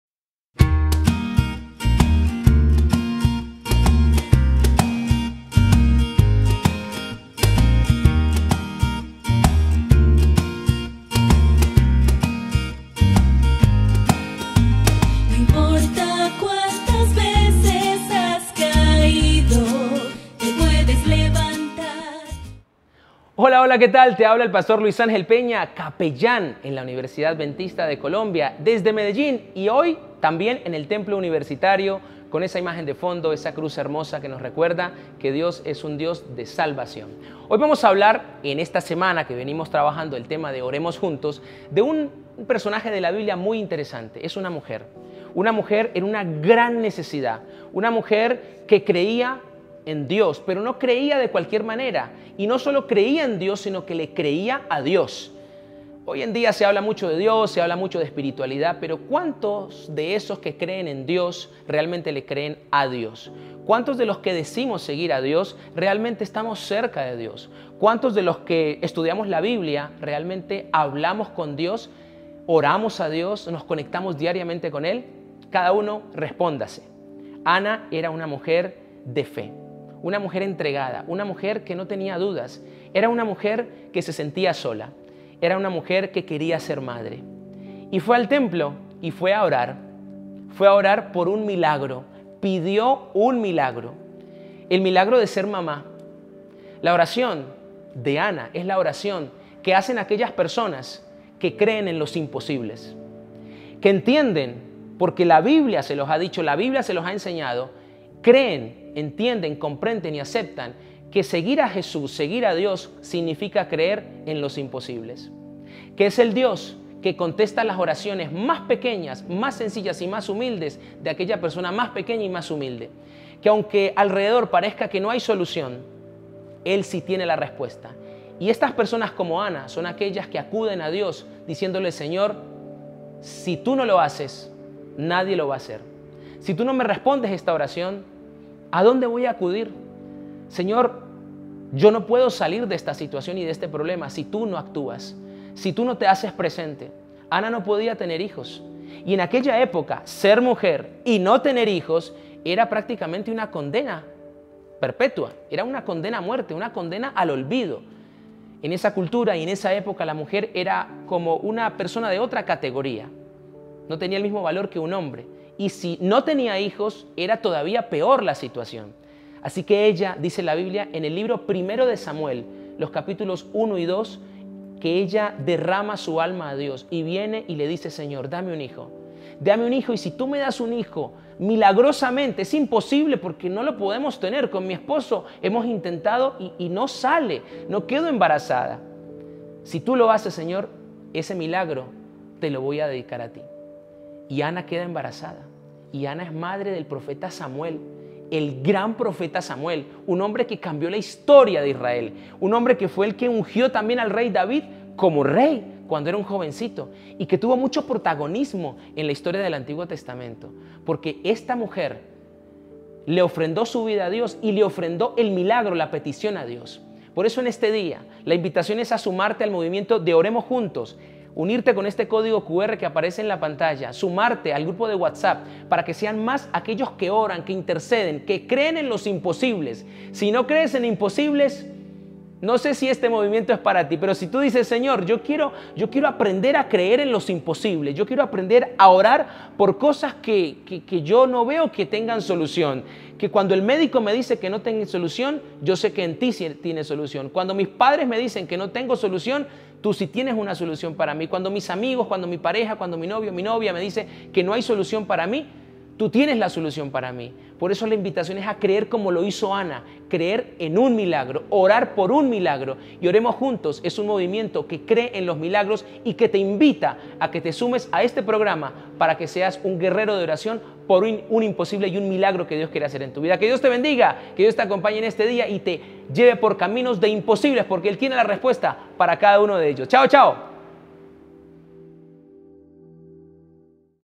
Oremos Juntos - Capellanía UNAC (14 de septiembre, Pr. Luis Ángel Peña)
Esta serie de 5 devocionales nos habla acerca de lo importante que es la oración en la Biblia y en nuestra vida. ¡Bienvenid@!

Sigue la cuenta de Instagram del Pr. Cristian Suárez